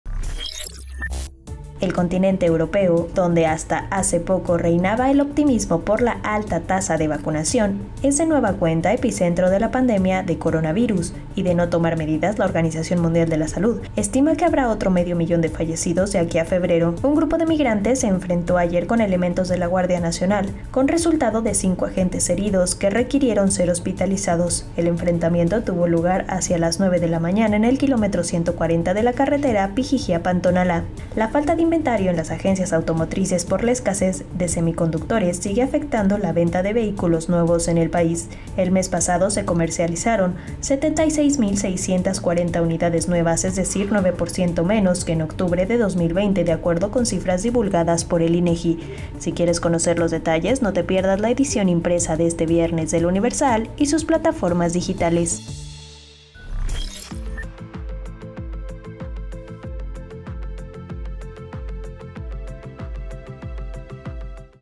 Europa, otra vez epicentro de Covid-19
OMS lamenta que la región es, “de nuevo, el epicentro” de la pandemia y podría provocar medio millón de muertes adicionales de aquí a febrero; Alemania sufre repunte